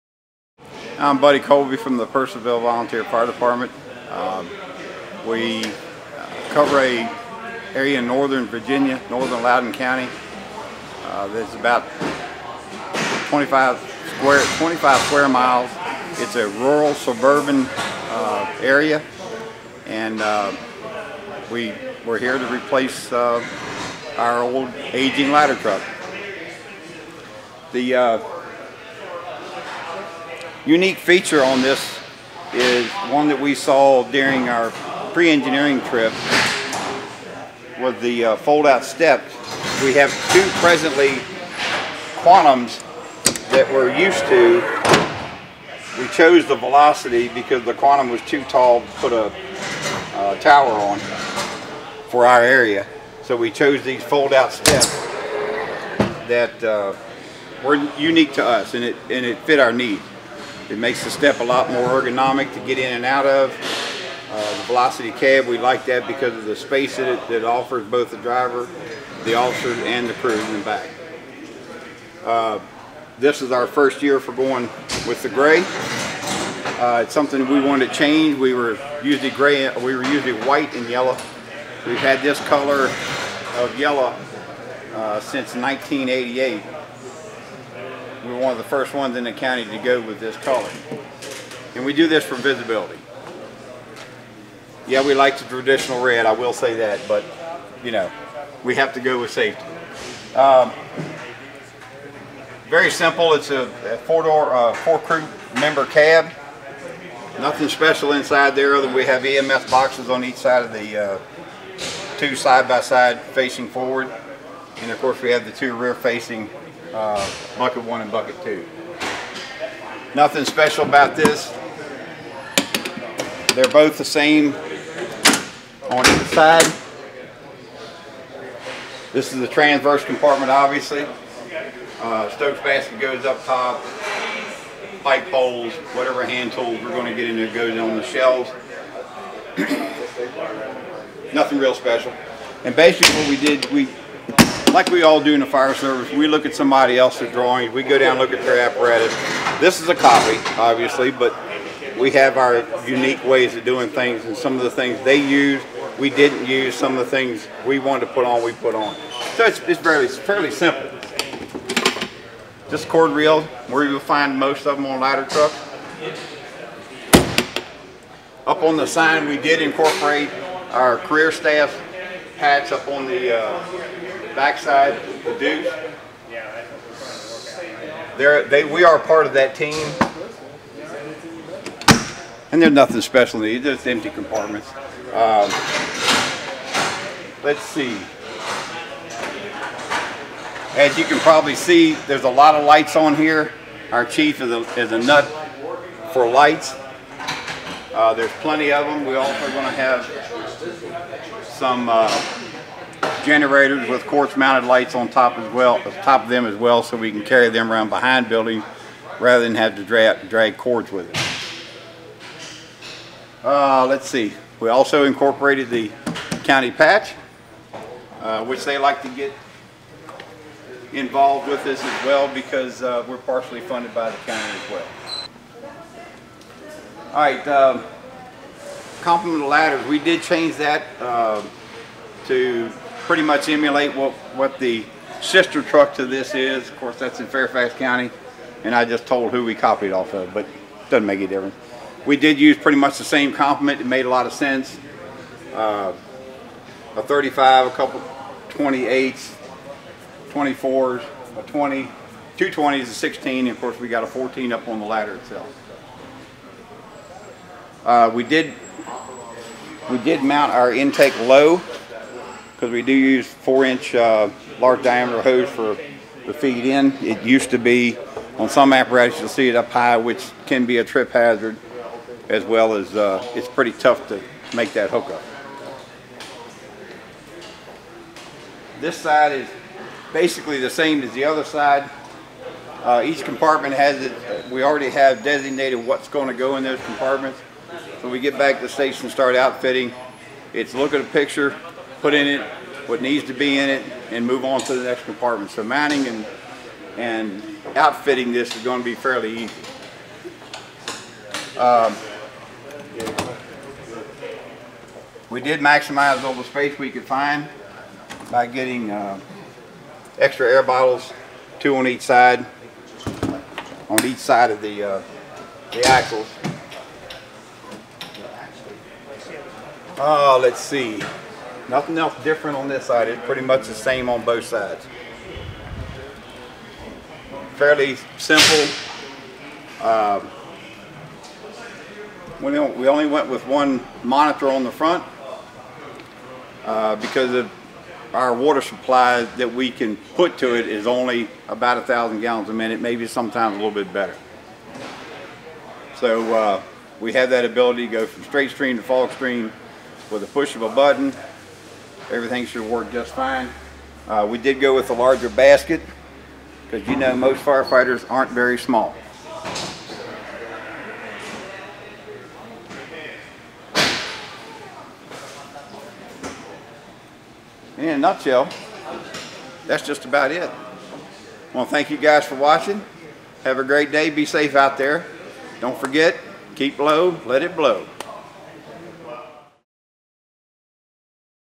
Purcellville, Velocity® 100’ Aerial Platform
Buddy Colby from the Purcellville Volunteer Fire Department, VA provides a walk around tour of their new platform with no pump. The department customized the Velocity by incorporating the power steps that they are accustomed to from their previous Quantum purchases. View specs below:

Chassis:
• Velocity chassis
• Front Axle: TAK-4® Independent Suspension
• Rear Axle: Meritor RT52-185
• GVW Rating: 76,800 lb
• Frame Rails: 13.38”
• Engine: DD13, 500 hp, 1650 lb-ft
• Transmission: Allison EVS 4500
• Wheelbase: 257”
• Overall Height: 146”
• Overall Length: 561.25”
• 6-person cab seating
• Side Roll & Frontal Impact Protection
• Bumper: 16” Extended
• Lift & Tow Package

Body and Aerial Device:
• Aluminum body
• 100’ Platform, 35 mph wind rating, 400 lb tip load
• Generator: Harrison Hydraulic, 15kW